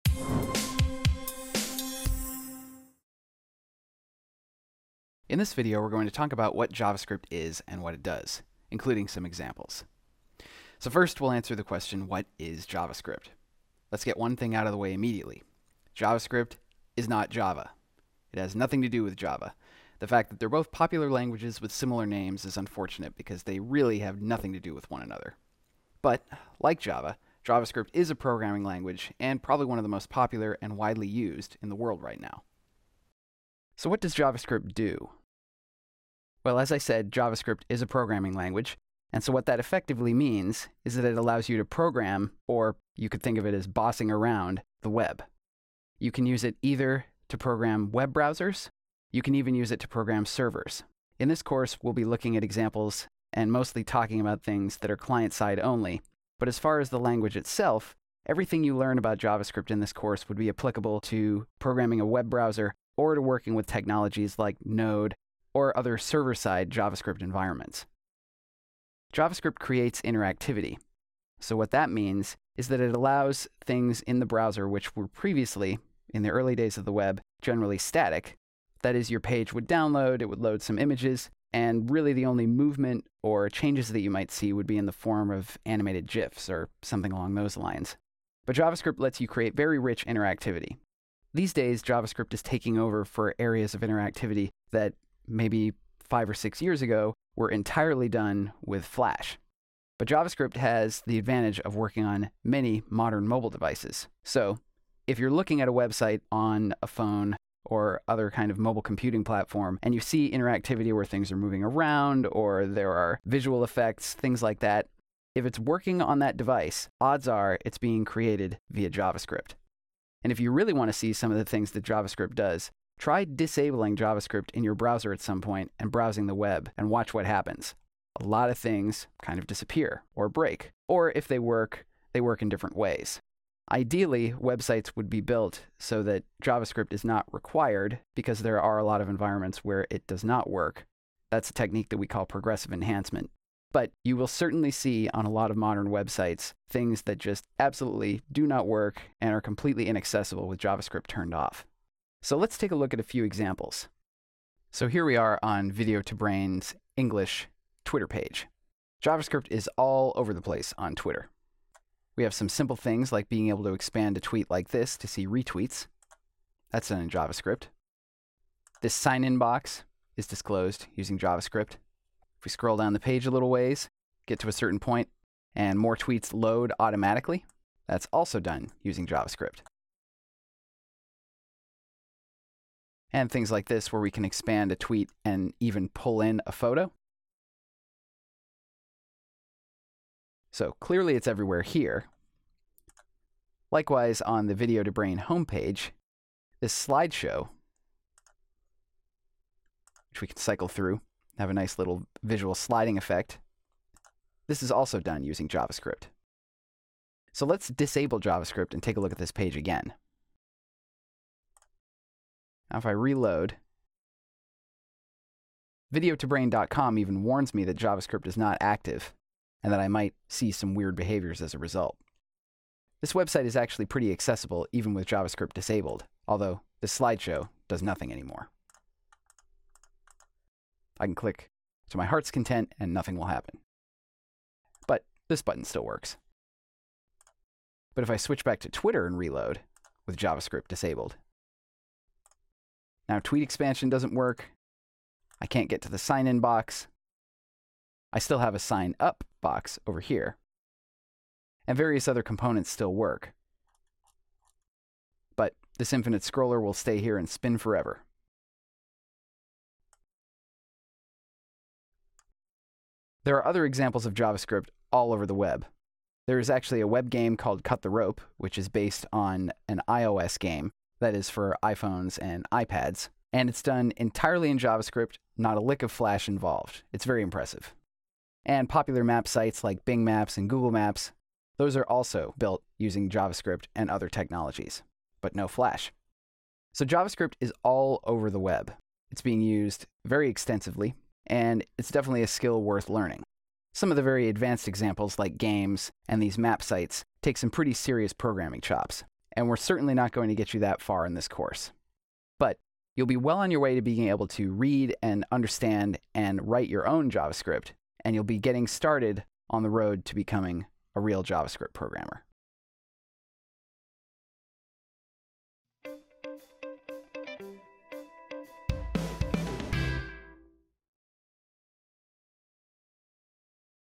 What Is JavaScript? What Does It Do?
In this free lesson from video2brain's course, Getting Started with JavaScript Programming, Joe Chellman explains what JavaScript is and what it does. He'll discuss JavaScript in general and look at specific examples from actual websites. For more information on this course, please